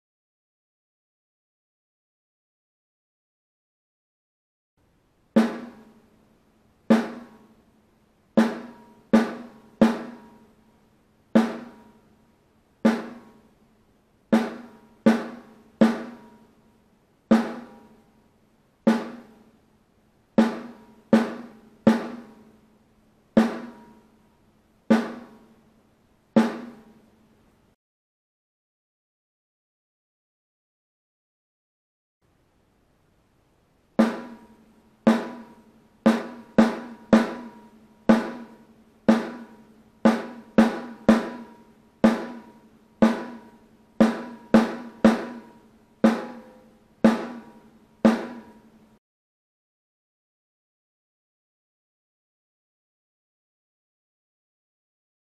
Lesson 4 - Ejercicio 3 (40/60 B.P.M.)
Snare Drum Method by Vic Firth. Aquí os dejo una interpretación de este ejercicio para el acercamiento a la caja